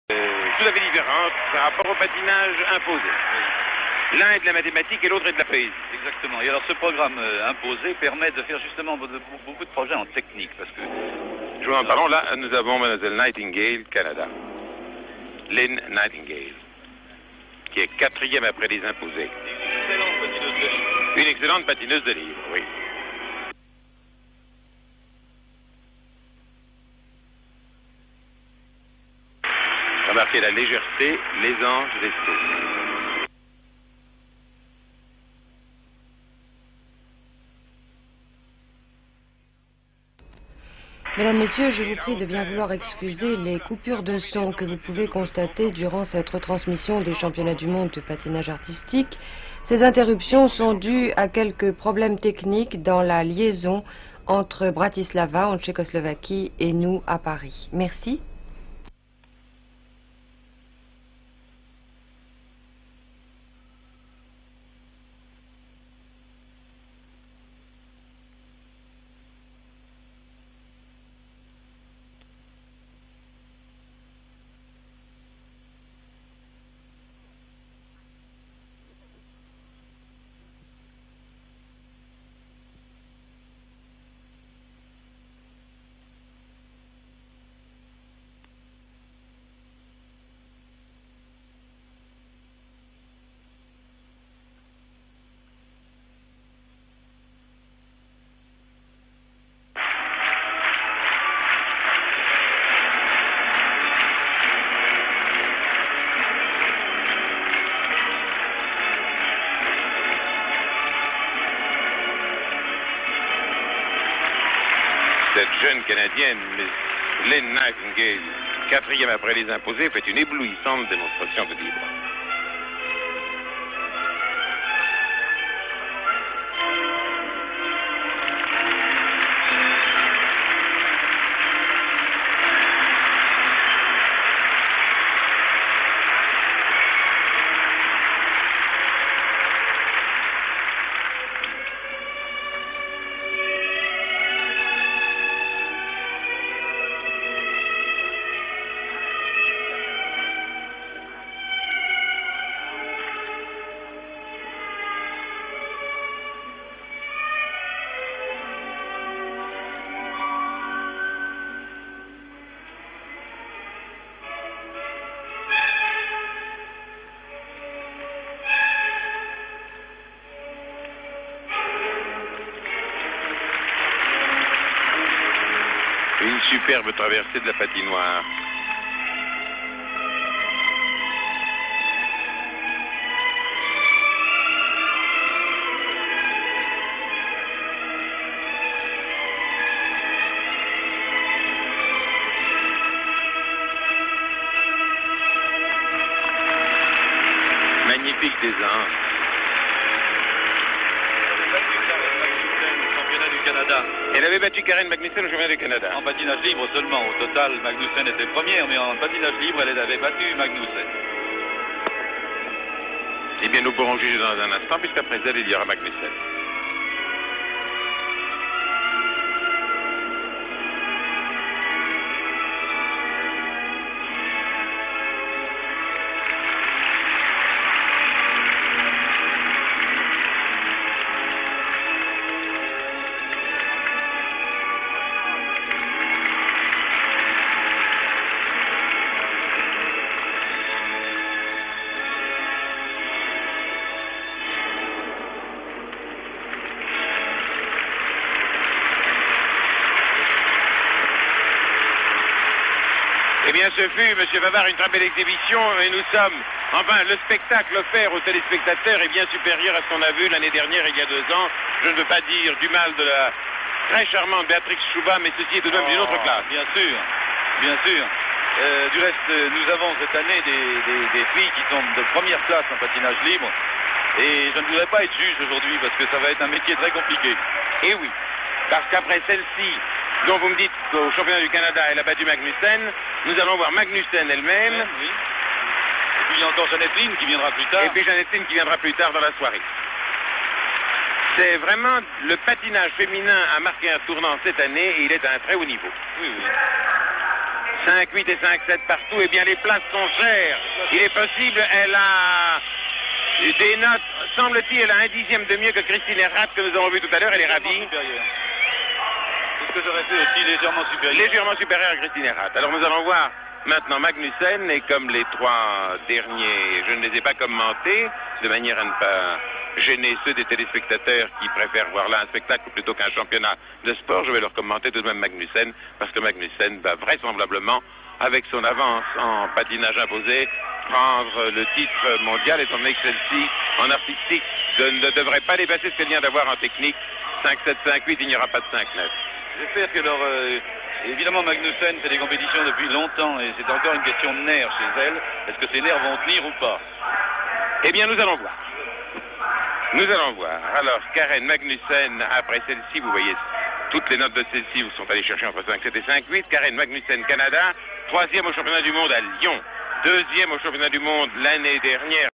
Lynn Nightingale 1973 World Figure Skating Championships LP
Lynn Nightingale of Canada, skating brilliantly in her international début at the 1973 Worlds in Bratislava. There is such utter joy and bravura to her skating here. She finished 10th at this event but was 4th in the inaugural short program and THIRD in free skating behind only Karen Magnussen and Janet Lynn. Her marks for this skate are really extraordinary considering this was her first major international!

My apologies for the sound kerfuffle at the beginning - it's the original broadcast - but the last two thirds of her performance and the kiss and cry are all OK.